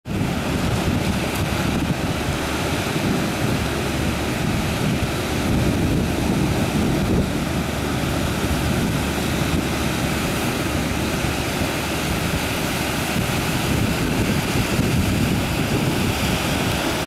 Peaceful moment waves crashing
Nice quiet peaceful moment at the North Shore Hawaii Dec 8, 2012